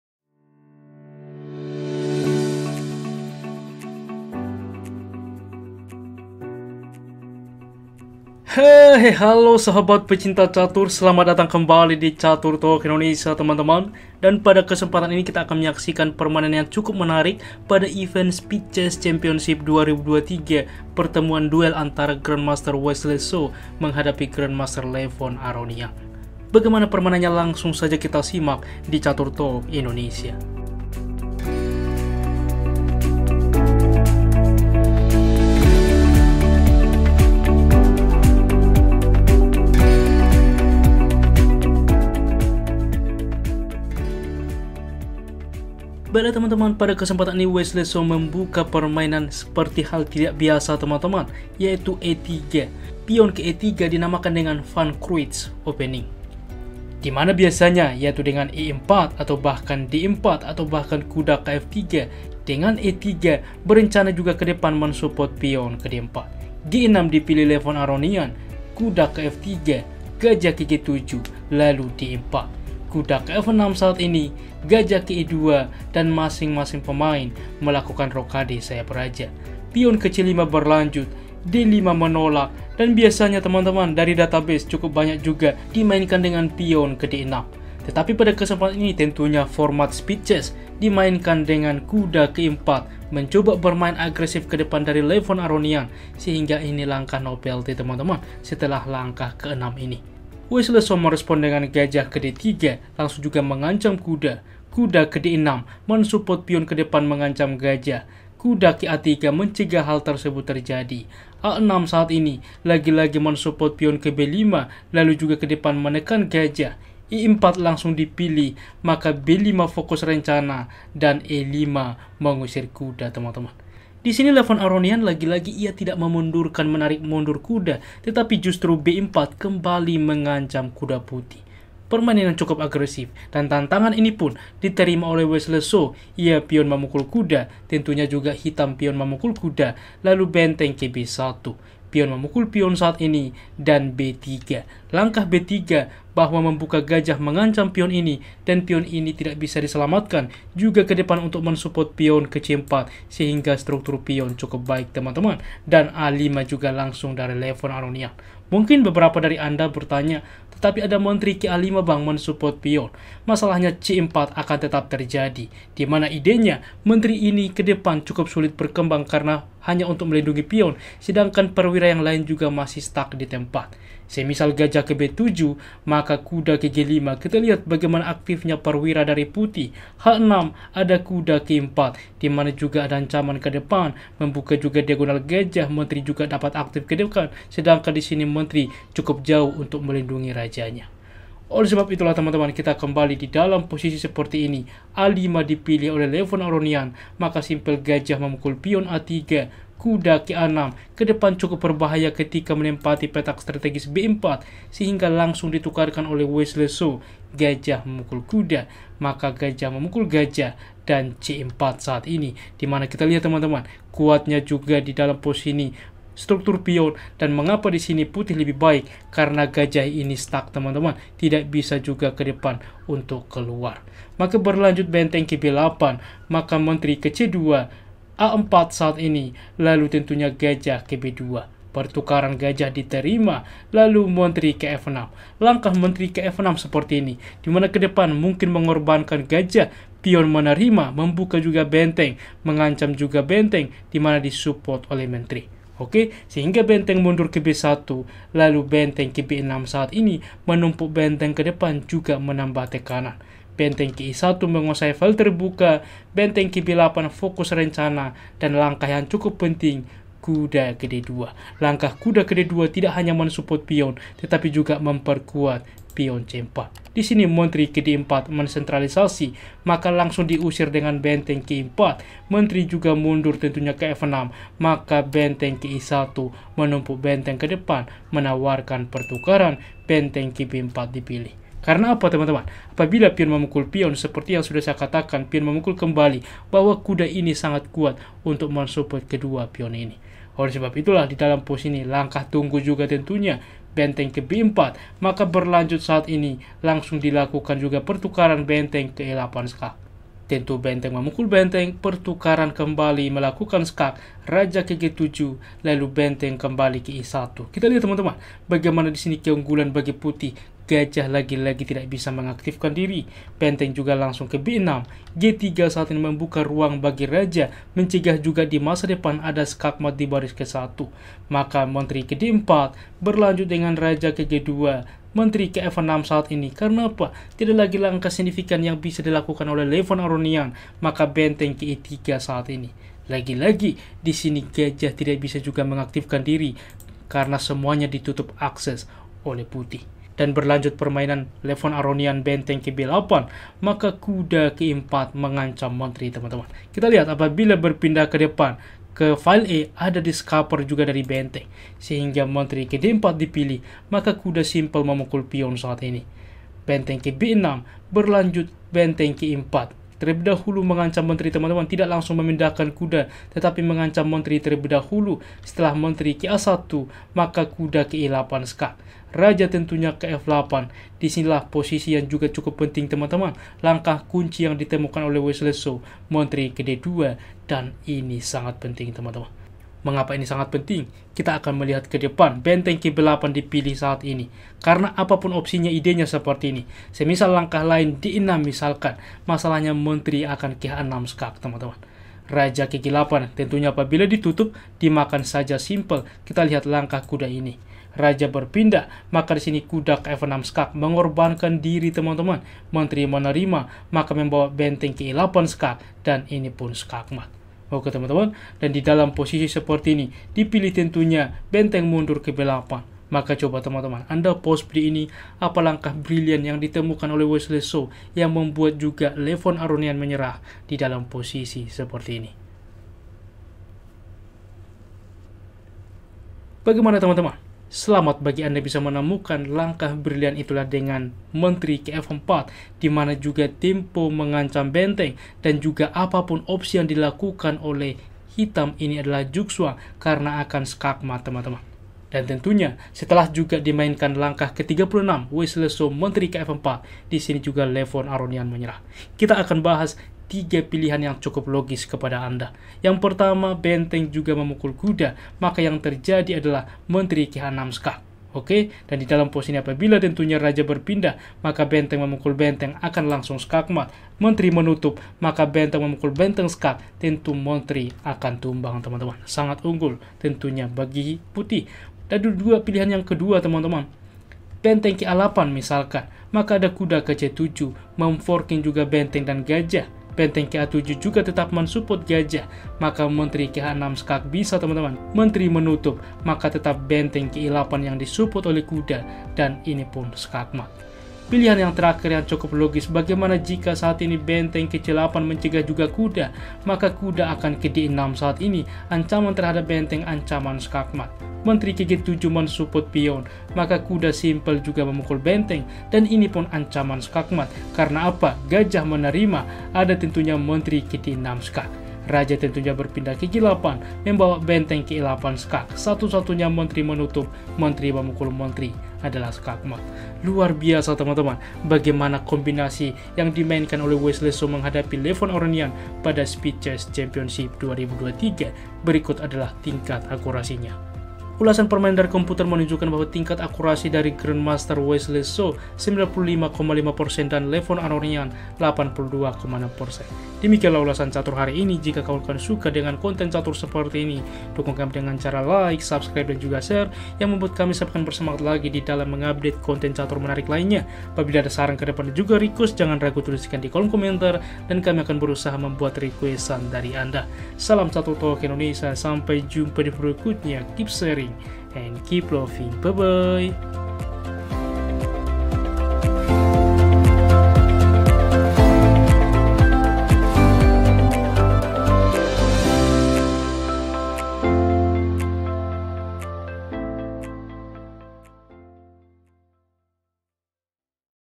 KEREN 👏 KOMBINASI WESLEY SO ❗️
PGN Game ! Wesley So Vs Levon Aronian
[Event "Speed Chess Championship 2023"]
[Site "Chess.com"]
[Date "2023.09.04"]
[Round "01-05"]
[White "So, Wesley"]
[Black "Aronian, Levon"]
[WhiteElo "3070"]
[BlackElo "3038"]
[TimeControl "300+1"]

1. e3 g6 2. Nf3 Bg7 3. d4 Nf6 4. Be2 O-O 5. O-O c5 6. d5 Ne4 7. Bd3 Nd6 8. Na3
a6 9. e4 b5 10. e5 b4 11. exd6 bxa3 12. Rb1 exd6 13. b3 a5 14. Bxa3 Na6 15. Bxa6
Bxa6 16. c4 Rb8 17. Qc2 a4 18. Bb2 Bxb2 19. Rxb2 Qf6 20. Rbb1 Rb6 21. Rfe1 Rfb8
22. Nd2 Qd4 23. Re4 Qf6 24. Rbe1 Rb4 25. Re8+ Rxe8 26. Rxe8+ Kg7 27. Re1 Rb6 28.
g3 Qd4 29. Kg2 Qf6 30. Re3 Rb8 31. Ne4 Qd4 32. Nxd6 Rb6 33. Re4 Qa1 34. Ne8+ Kf8
35. Qd2 Rb8 36. Qf4

Rekomendasi Produk (Catur, Ngonten, Workout, Fashion, Grooming) (Koleksi Afiliate Shopee)